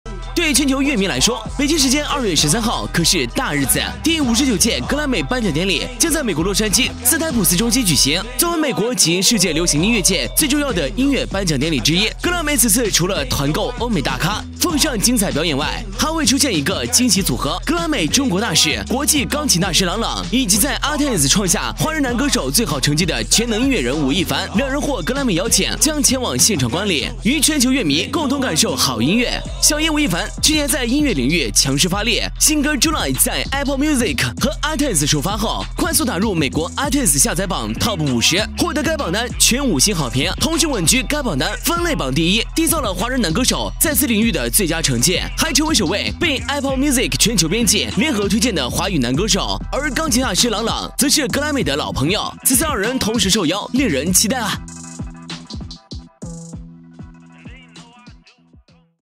【3年前】吳亦凡受邀格萊美 將與朗朗現場觀禮頒獎典禮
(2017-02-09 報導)

對全球樂迷來說，北京時間2月13號可是大日子，第59屆格萊美頒獎典禮將在美國洛杉磯斯臺普斯中心舉行。作為美國及世界流行音樂界最重要的音樂頒獎典禮之一，格萊美此次除了“團購”歐美大咖、奉上精彩表演外，還會出現一個驚喜組合——格萊美中國大使、國際鋼琴大師郎朗，以及在itunes創下華人男歌手最好成績的全能音樂人吳亦凡，兩人獲格萊美邀請，將前往現場觀禮，與全球樂迷共同感受好音樂。